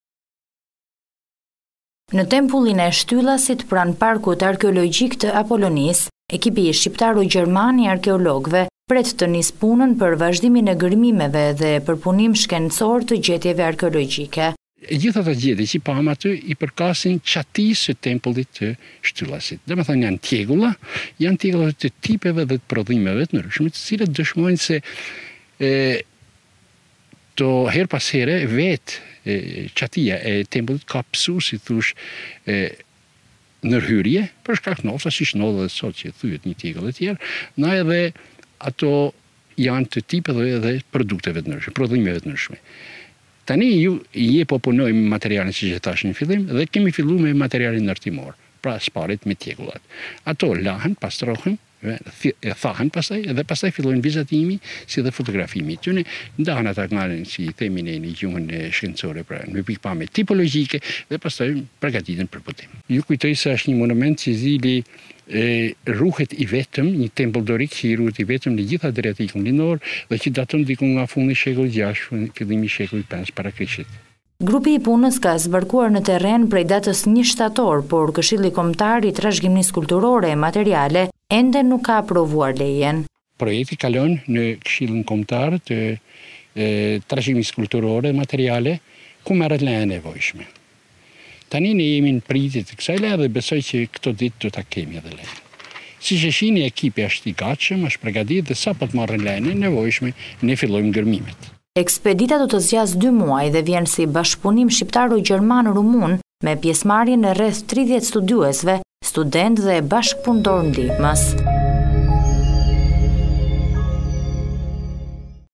Gërmimet arkeologjike në Shtyllas të Apolonisë peng i burokracisë, nuk aprovohet leja
reportTV lajmeshqip lajme 30 arkeologë e studiues shqiptarë, gjermanë e rumunë presin prej datës 1 shtator të nisin gërmimet arkeologjike në Tempullin e Shtyllasit, pranë Apolonisë. Pengesë për punën e tyre është bërë Këshilli Kombëtar i Trashëgimisë Kulturore e Materiale, i cili ende nuk ka aprovuar lejen për gërmime. Rreth kësaj problematike flet professor doctor, Bashkim Lahi që drejton këtë ekspeditën.